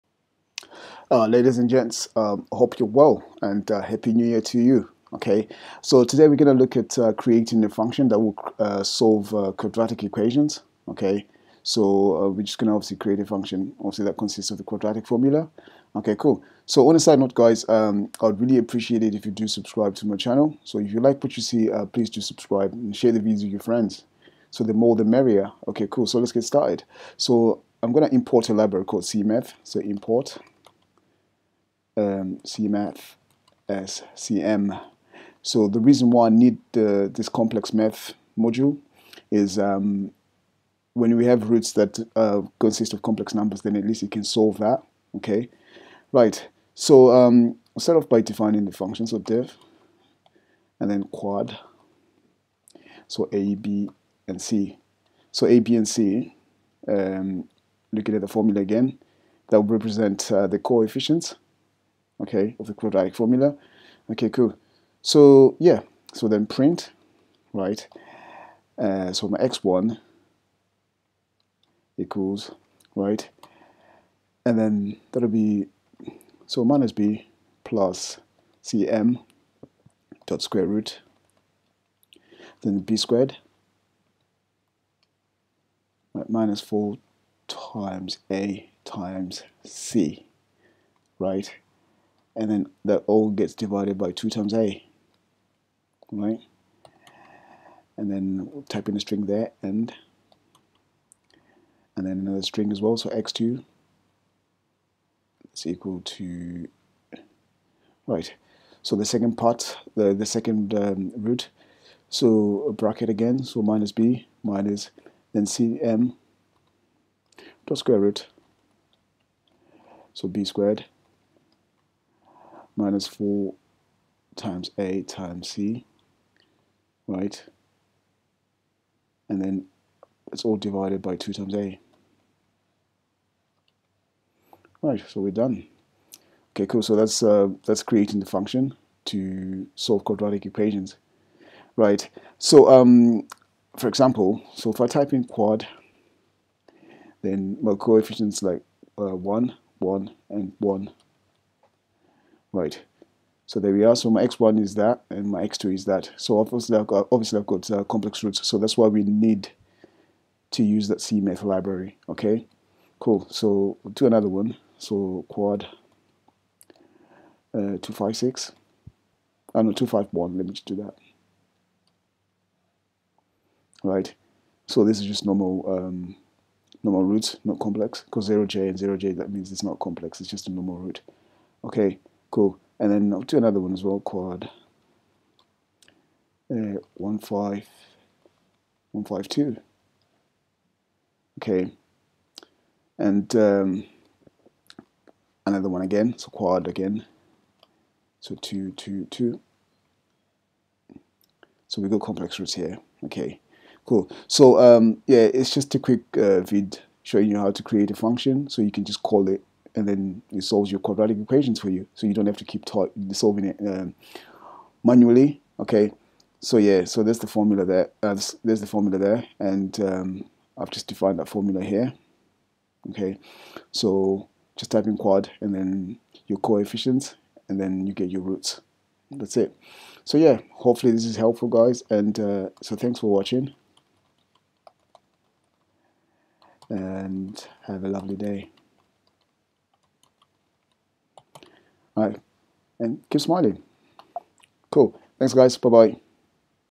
How to create a function to solve quadratic equations with Python
Import the cmath module as that is needed to solve complex roots
def function
Call the function to solve different quadratic equations.